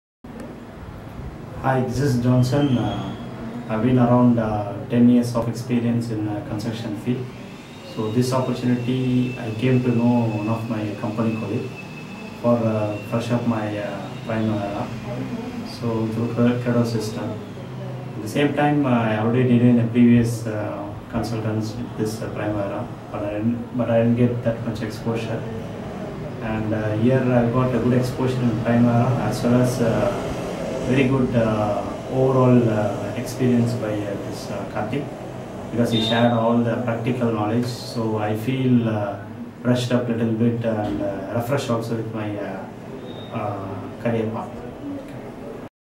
CREDO SYSTEMZ - PRIMAVERA P6 TRAINING - Arokya Johnson - Video Review & Feedback
The above is a video review and feedback given by a candidate who has completed the training with us, CREDO SYSTEMZ.

All the opinions and feedback given in the video are own words of the candidate.

For more details :

Primavera Training in Chennai || Primavera P6 Training in Chennai